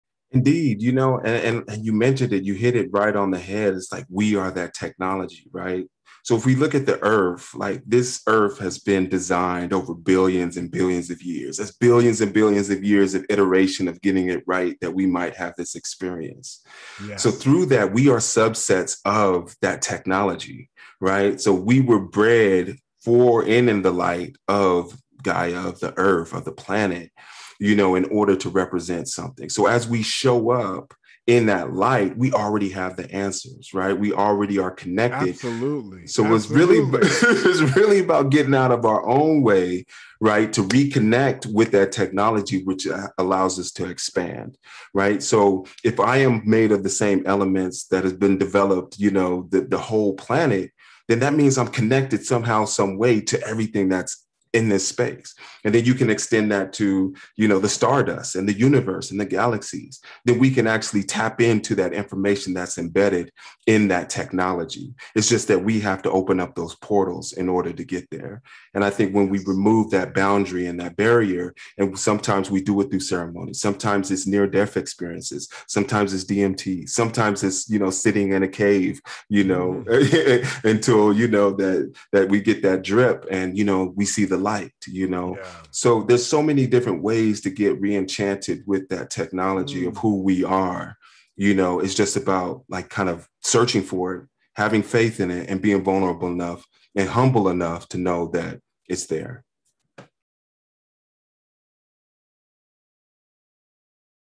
MARQUES ANDERSON "WE ALREADY ARE CONNECTED"
Marques Anderson is a cyborg anthropologist, innovator and world builder. Seeking to be globally impactful, he created the World Education Foundation in 2009. The WE Foundation’s mission and aim focuses on bridging the gap between academia, technology and implementation in local communities around the world. Since its conception, he has had the opportunity to travel to over 80 countries, listening for challenges at the intersection of humans, society and ecology. He is currently mobilizing his experience and expertise to help multiple businesses, industries, and communities re-imagine the future. Having had a five-year career as an elite athlete in the NFL, Marques has embodied and experienced team work, discipline, grit and the passion to be a champion in multiple fields.